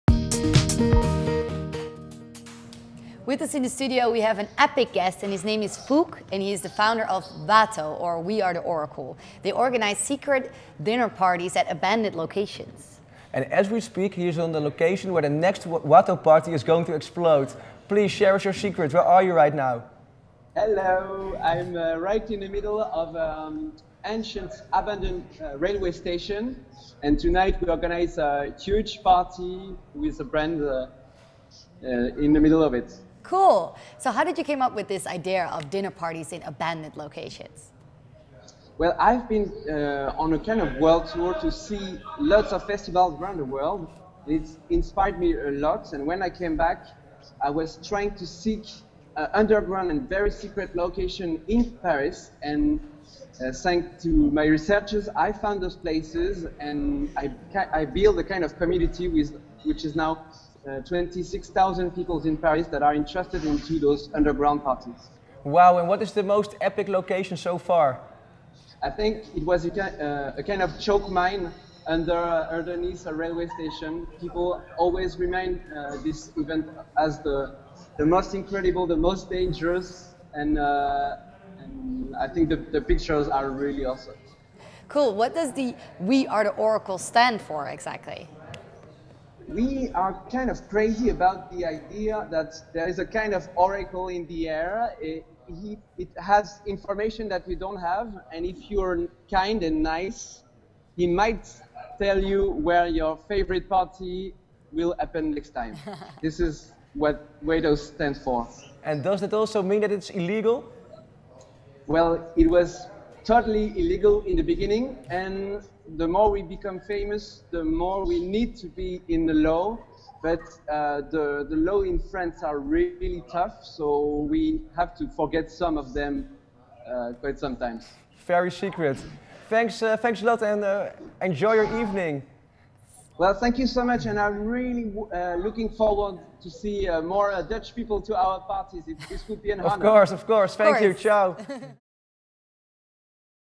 Dine At Paris’ Craziest & Illegal Locations
In Paris there is a way to do so with secret dining organized by We are The Oracle (WATO). Founder Foulques Jubert started WATO after he visited festivals all around the world. Discovering all kinds of abandoned venues around Paris Foulques decided to make use of them. The name originated from Foulques and his believe in an all knowing oracle and if you listen carefully you may be rewarded.
These dinner parties are secret for a reason because they are not fully legal. We wanted to know why they are illegal and how does the whole secret dining system work? We got a chance to speak to Foulques just before another WATO party, in an abandoned Paris Railway station.